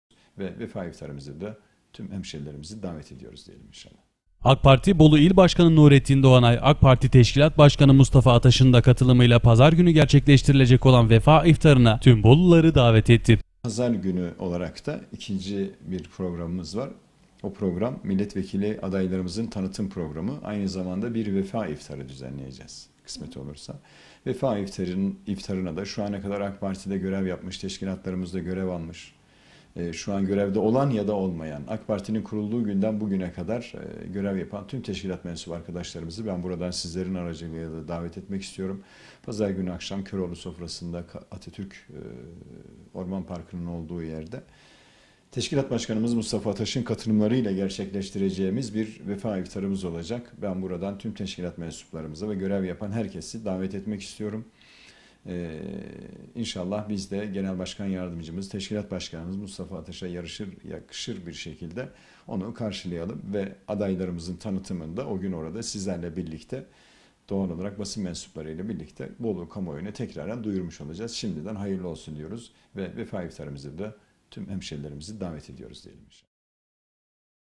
AK Parti’den vefa iftarı (26.05.2018)
AK Parti Bolu İl Başkanı Nurettin Doğanay, AK Parti Teşkilat Başkanı Mustafa Ataş’ın da katılımıyla Pazar günü gerçekleştirilecek olan vefa iftarına tüm Boluluları davet etti.